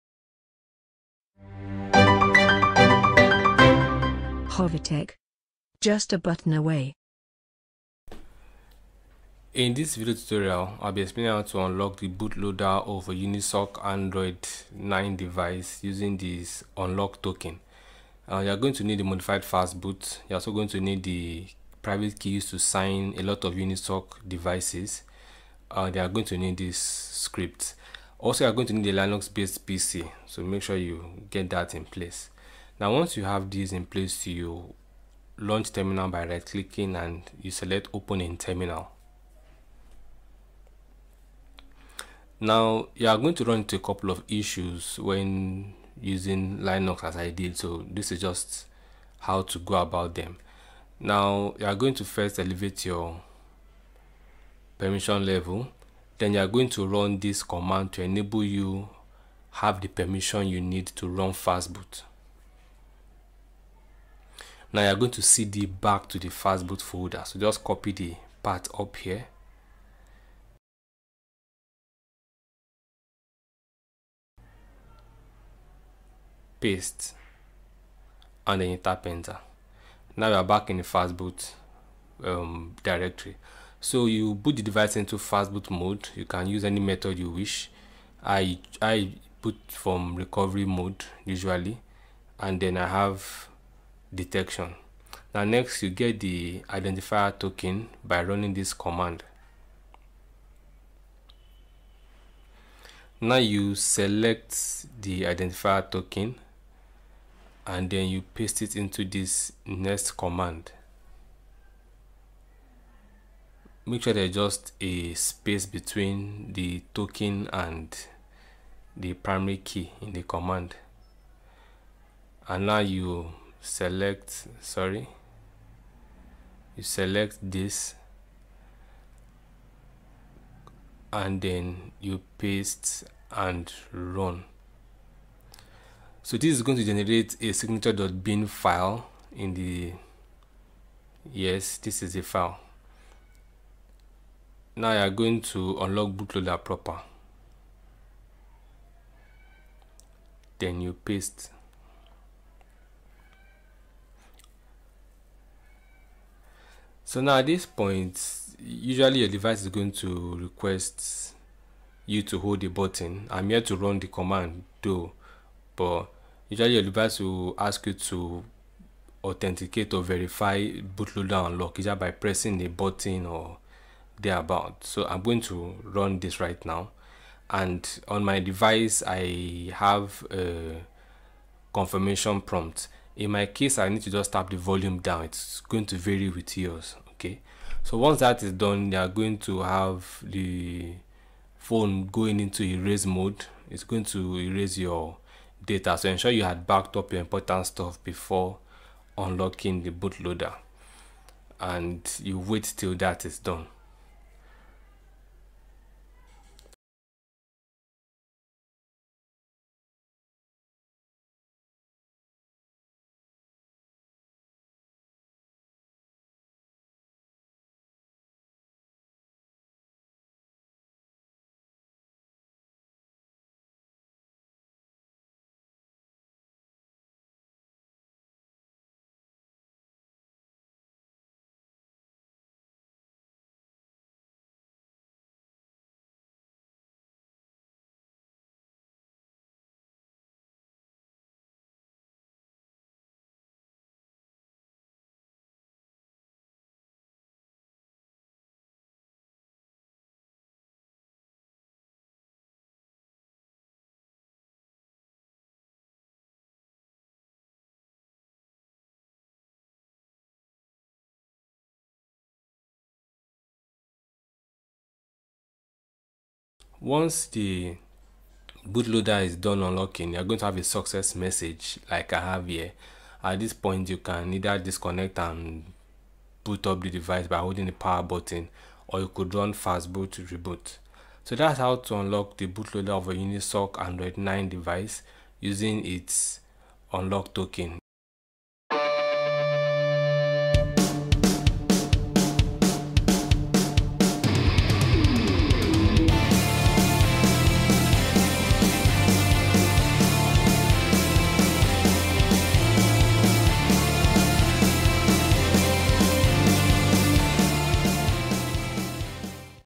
How to unlock Unisoc (SPD) bootloader using Identifier Token
This guide is for Unisoc Android 9 (Pie) devices which require a bootloader unlock binary. It'll also work for you if our OEM uses the same Private Key.

You'll need a Linux-based PC and the modified fastboot used in this guide.
Most Unisoc OEMs we've tested appear to use the same Private Key (used in the guide). If yours doesn't then you'll need their private key to unlock booloader